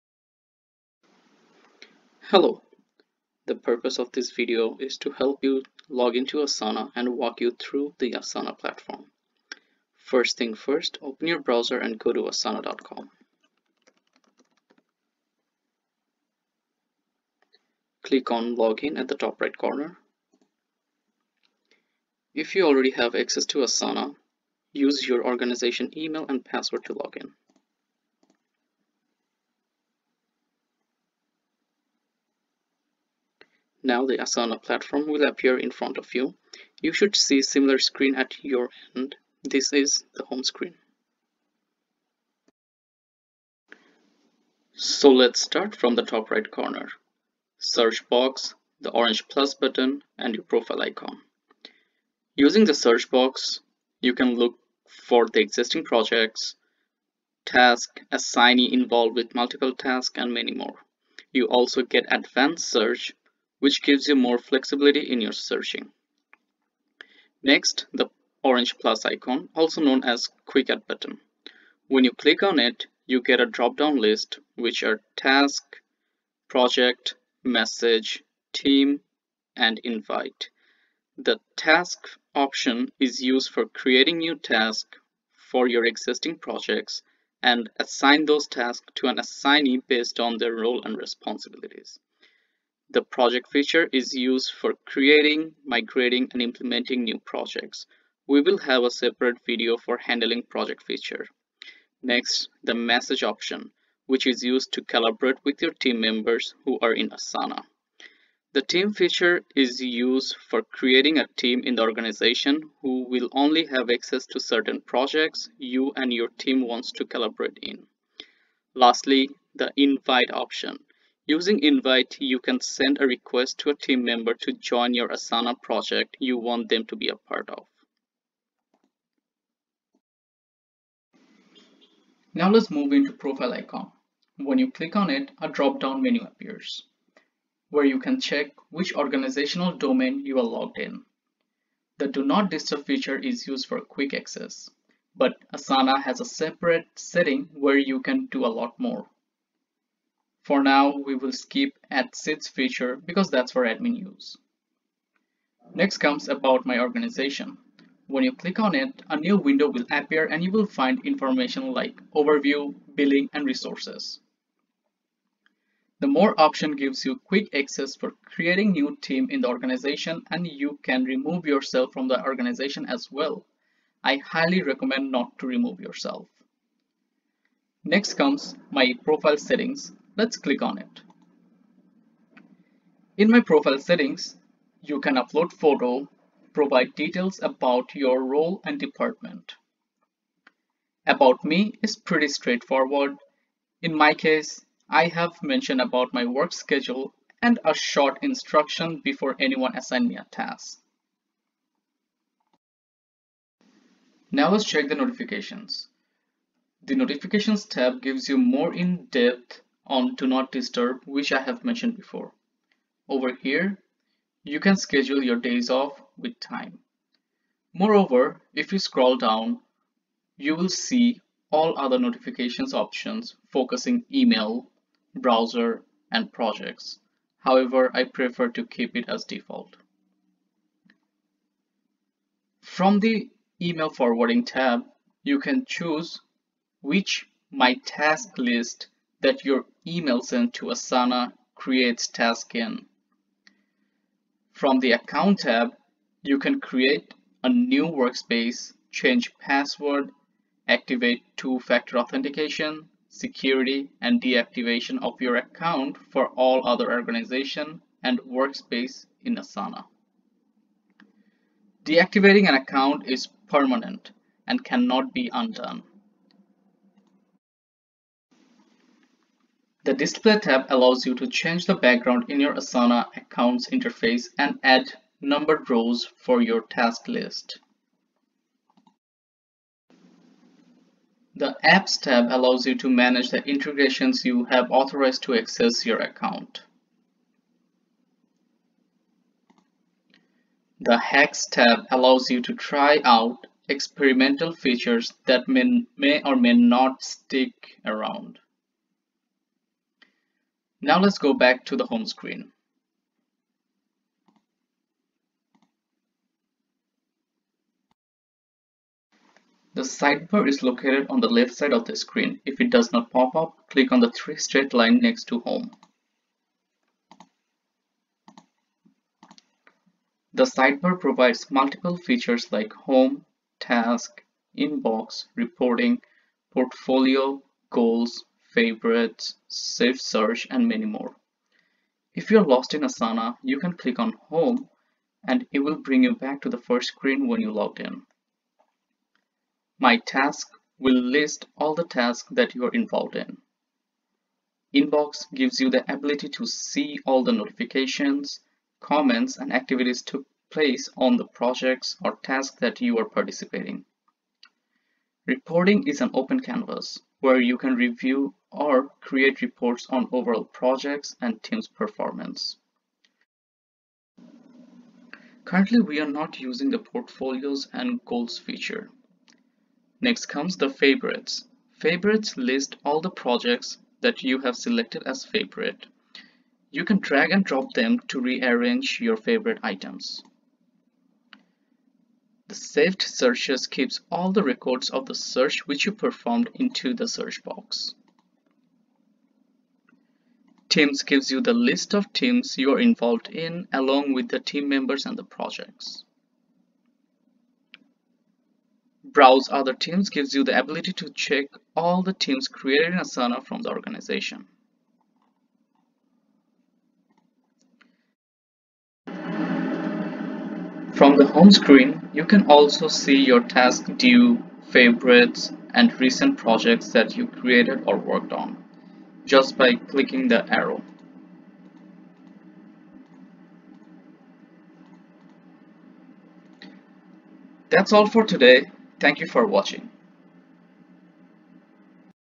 Asana Brief Overview
In this video, you will learn how to log in to Asana and a short overview of the Asana platform.